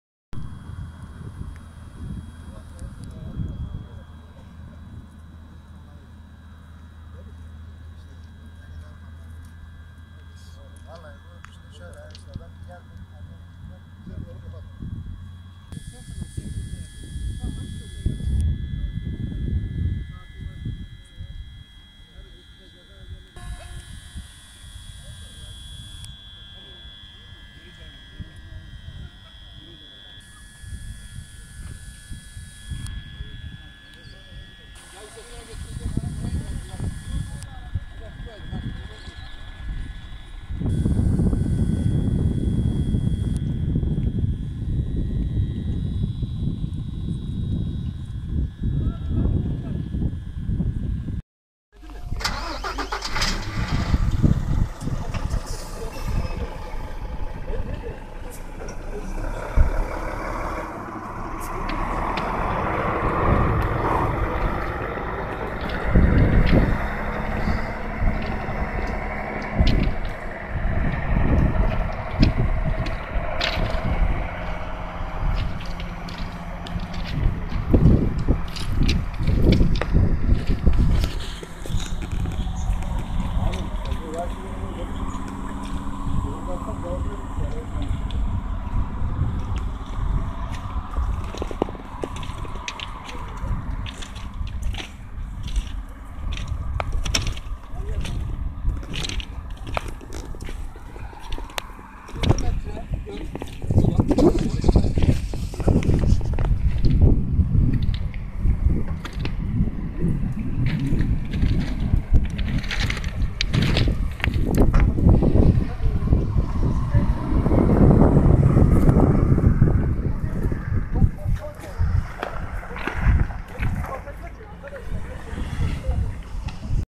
TIR TREN YOLUNA GİRDİ FACİAYADAN DÖNÜLDÜ
Kahramanmaraş'ta kontrolden çıkan TIR tren raylarına takılarak durabildi. Bu esnada hareket halinde olan trenin makinistinin durumu fark etmesiyle olası bir faciadan dönüldü.